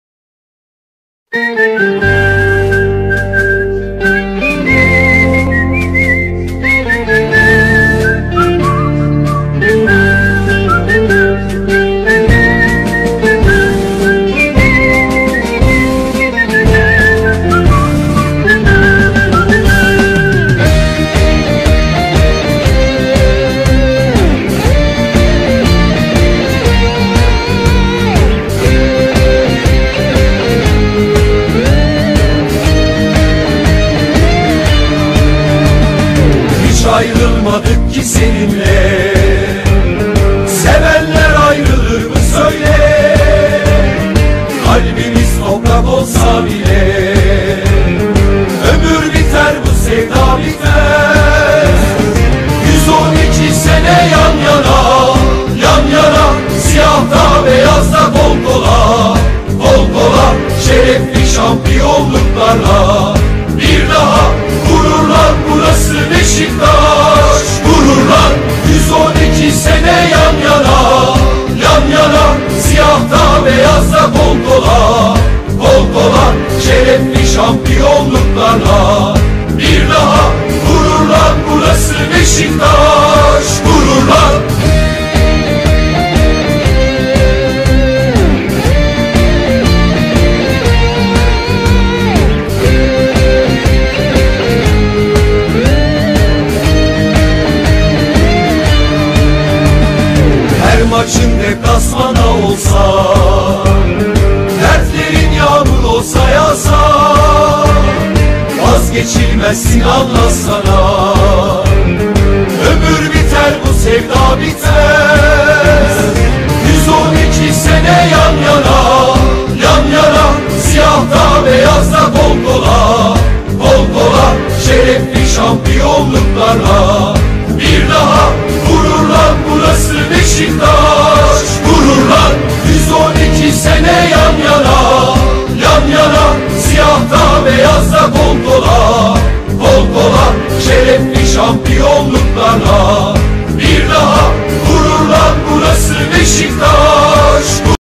Pas çalışması ve yön değiştirme
Beşiktaş Fatih Futbol Okulu olarak geleceğin kartallarıyla bu hafta futbolun hücum prensiplerinden pas ve destek konusunu işledik. Yaptığımız 3 farklı varyasyonu olan çalışmada pası alacak oyuncunun boşa kaçması, yön değiştirmesi ve dar alan oyunları gibi konuları pas çalışmasıyla yaptık